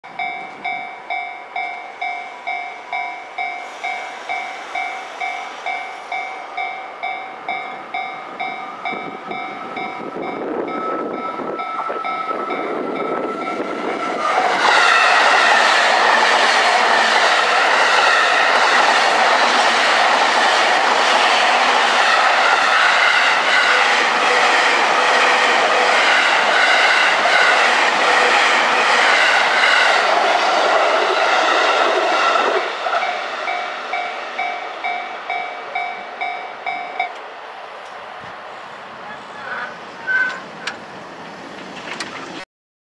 JR東海道本線E233系普通平塚行き&E257系特急踊り子東京行き 川崎付近の踏切通過
JR東海道本線川崎駅付近の踏切を通過するE233系U629編成の普通平塚行きと、E257系の特急「踊り子」東京行きです。撮影日は2022年4月16日です。

今回の動画は踊り子を撮影しようと思っていたら、下り普通にかぶられたという、ただそれだけの動画です。残念。